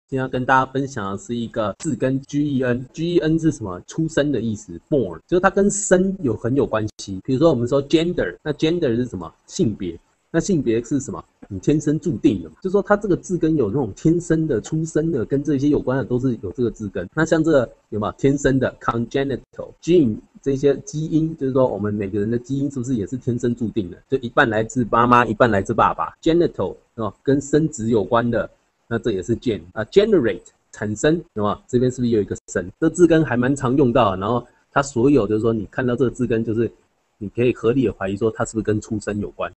【字根 gen】English4Formosa 字首字根推廣教材
mutagen (n.) 突變原

解說: 在遺傳學中, 使生物體內的遺傳訊息發生變化的物理或化學因子。生物若處於這些因子的作用下，發生突變的機會將高於在一般自然狀況中。(from Wikipedia)

在字首字根來看

mut 是代表 「改變」的字根; gen 是代表 「產生」 的字根
合起來就是「會使基因產生改變的化學因子」

相關單字:
carcinogen (n.) 致癌物質
mutant (adj.) 突變的